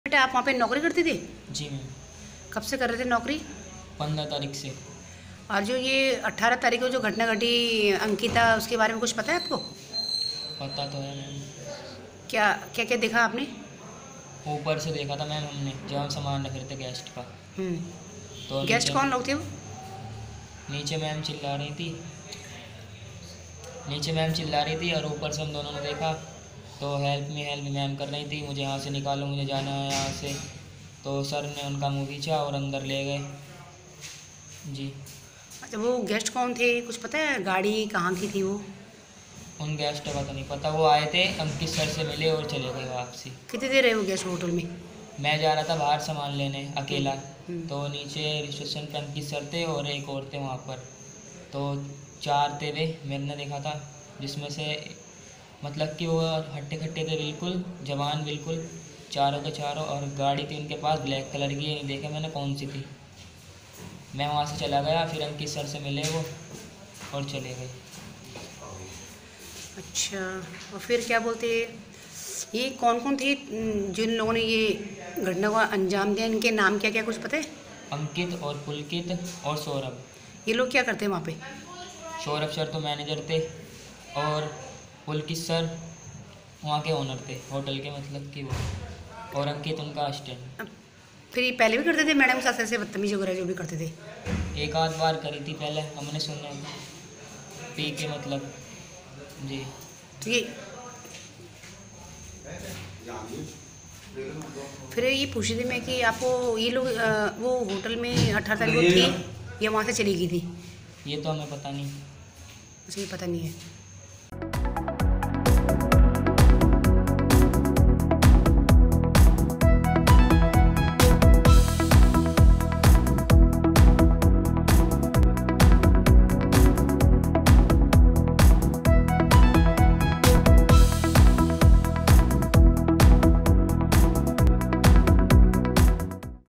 Ankita Case : रिसोर्ट के कथित पूर्व कर्मचारी ने बताई आंखों देखी, उस दिन रिसोर्ट में .... Exclusive
उस दिन रिसोर्ट में 4 लोग आए थे, मैम हेल्प हेल्प चिल्ला...